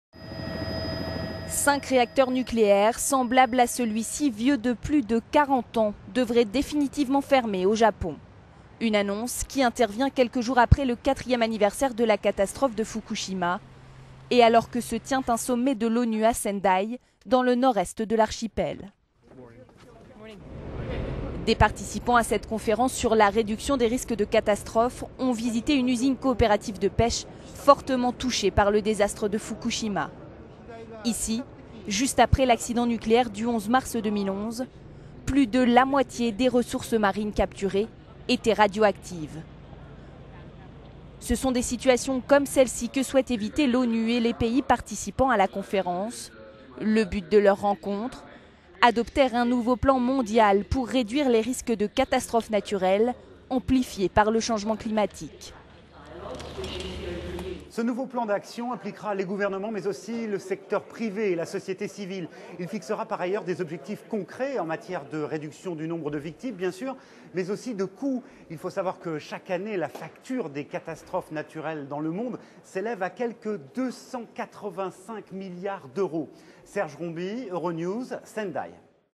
La réduction des risques de catastrophes naturelles au centre des préoccupations mondiales
Cinq réacteurs nucléaires, vieux de plus de 40 ans, devraient définitivement fermer au Japon. Cette annonce intervient quelques jours seulement après le quatrième anniversaire de la catastrophe de Fukushima. Elle répond à un plan adopté en 2013 par le gouvernement japonais et selon lequel, tout réacteur nucléaire en fonctionnement depuis 40 ans doit soit être révisé, soit définitivement fermé.
Au Japon, tous les réacteurs nucléaires sont à l'arrêt depuis la catastrophe de Fukushima.